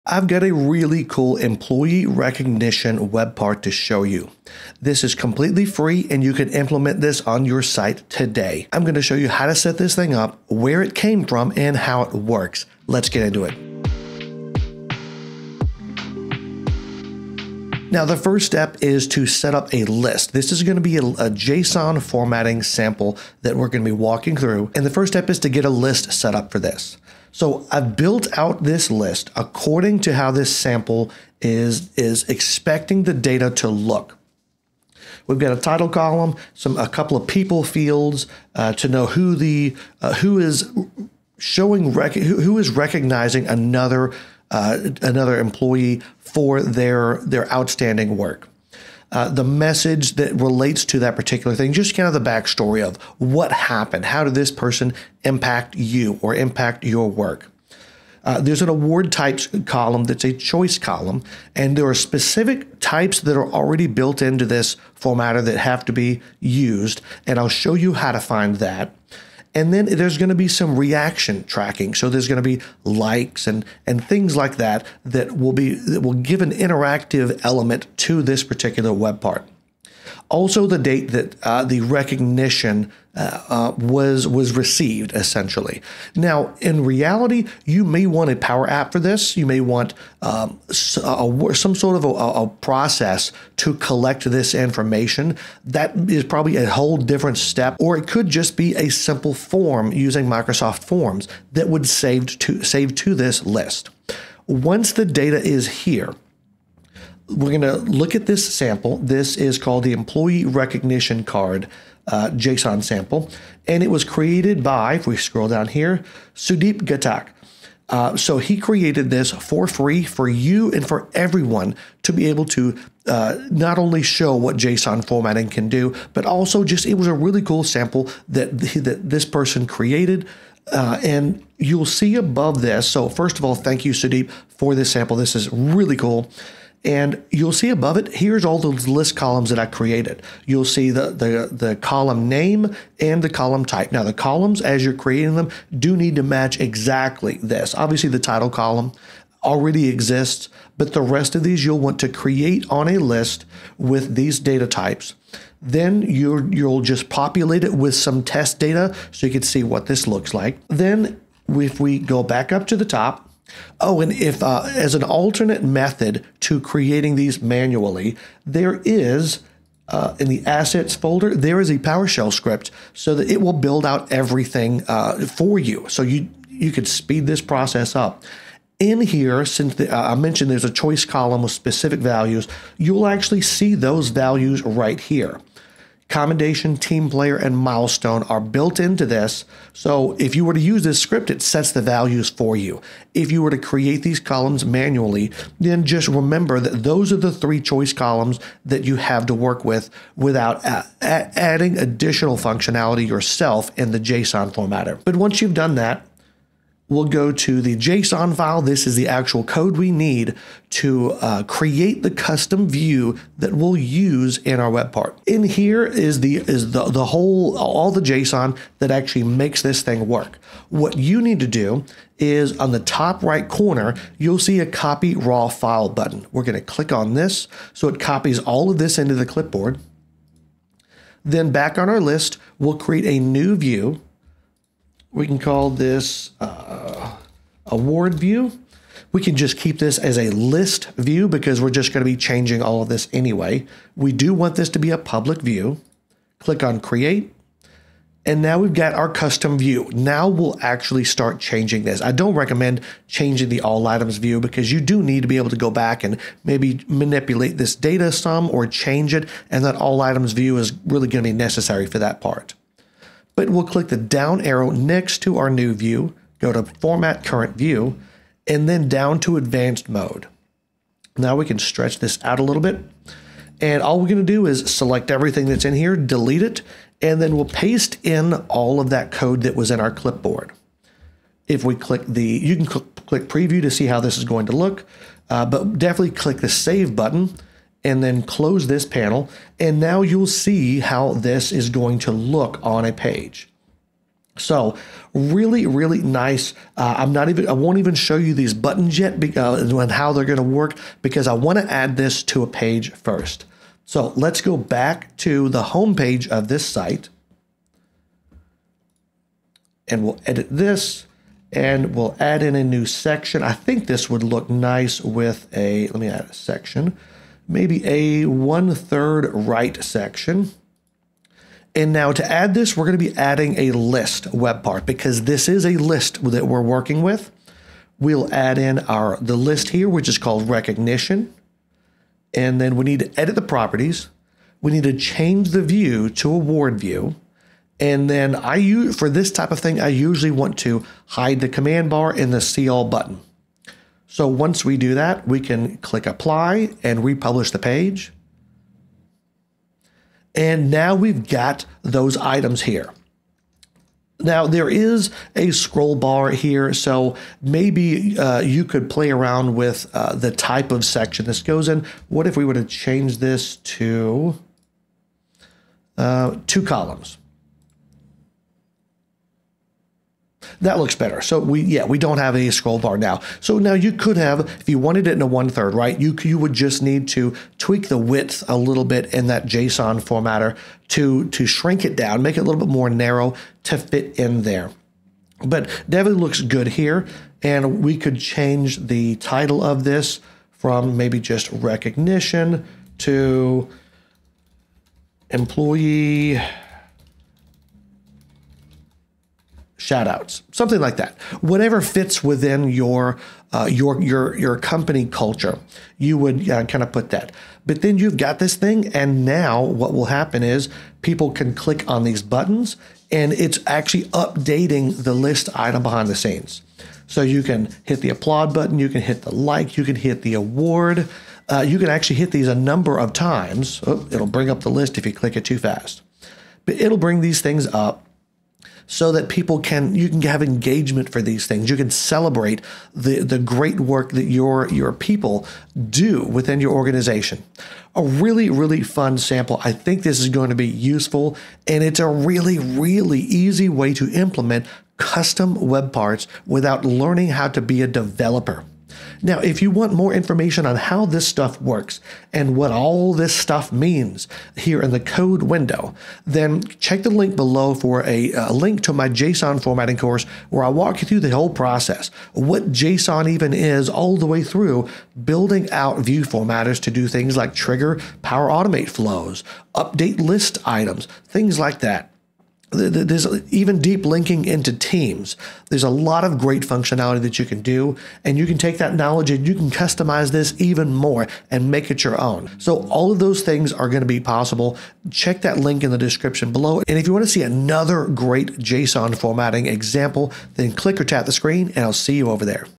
FREE Employee Shoutouts Solution For SharePoint
Here's how you can add an employee recognition solution to SharePoint.

About this video:
This video shows how to use JSON formatting in SharePoint to create a custom employee recognition system. It leverages out of the box SharePoint to let you shout out employees that deserve recognition.